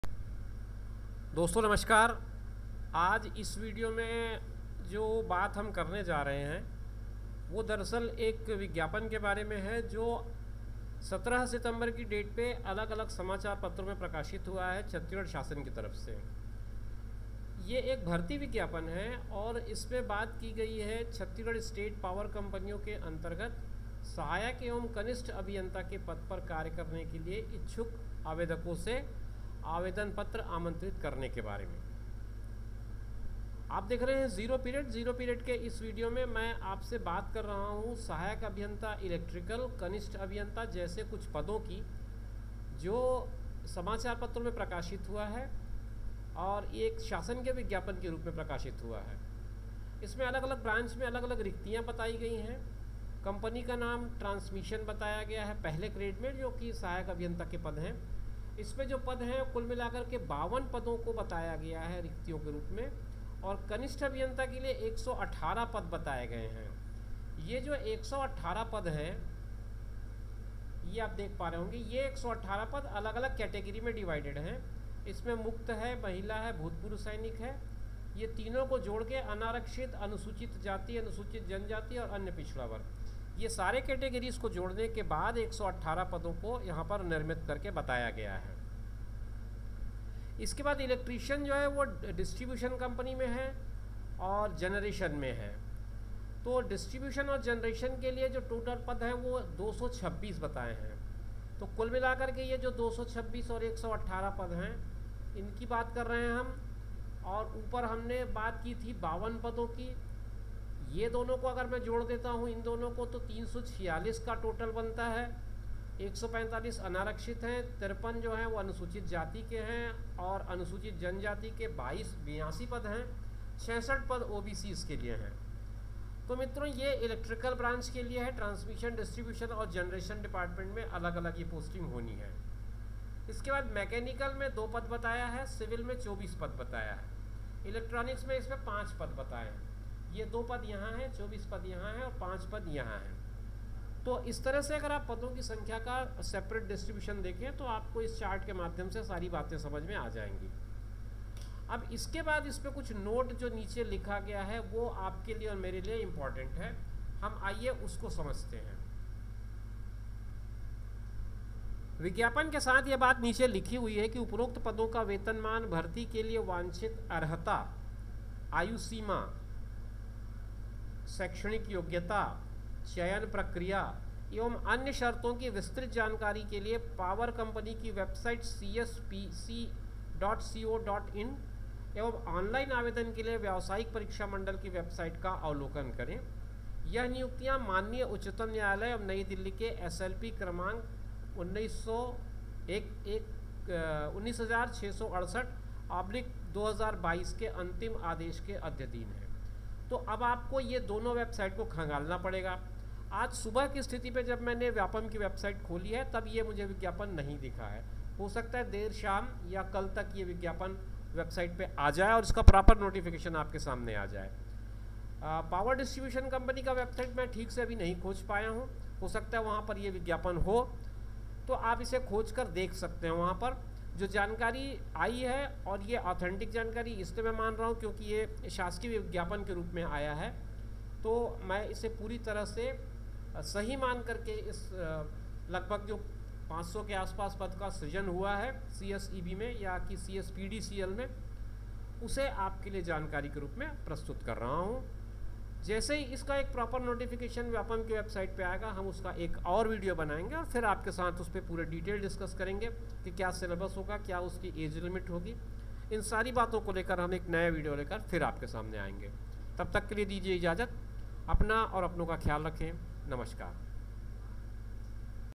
CSPDCL Recruitment 2023
छत्तीसगढ़ सरकार  की ओर से पावर कंपनी में इंजानियरों का भर्ती का विज्ञापन जारी किया गया है जिसके बारे में इस वीडियों में विस्तार से बात की गई है । कुल 429 पदों पर भर्ती की  जा रही है ।